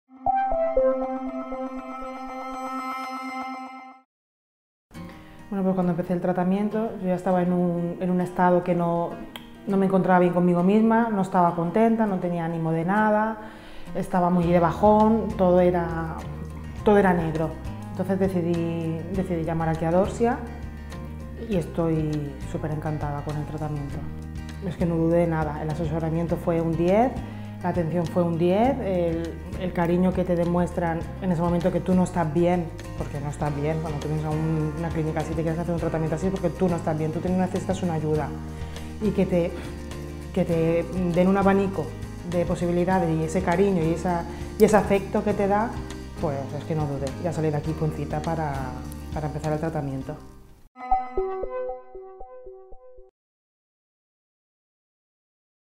María del Mar Ruiz - Clínicas Dorsia
María del Mar Ruiz nos cuenta su experiencia en clínica Dorsia Palma de Mallorca (Vía Alemania)